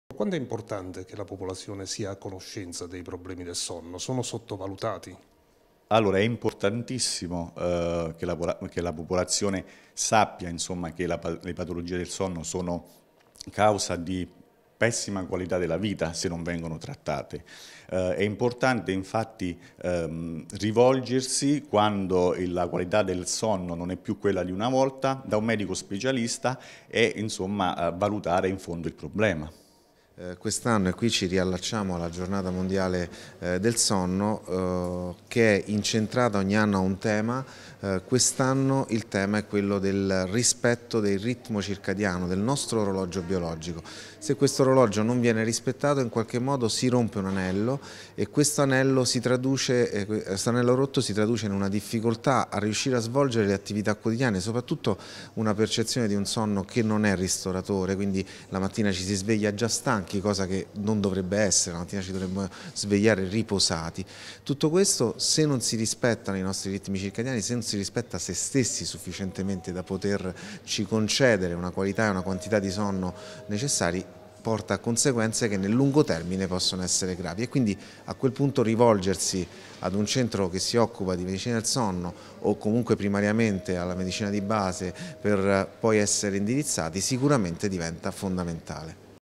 Giornata Mondiale del sonno
Apnee, sindrome delle gambe pesanti, russamento e narcolessia: sono solo alcuni dei distrurbi legati al sonno e che abbassano notevolmente la qualità di vita delle persone. Il rispetto del ritmo sonno-veglia diventa fondamentale. Ce ne parlano gli esperti del Neuromed.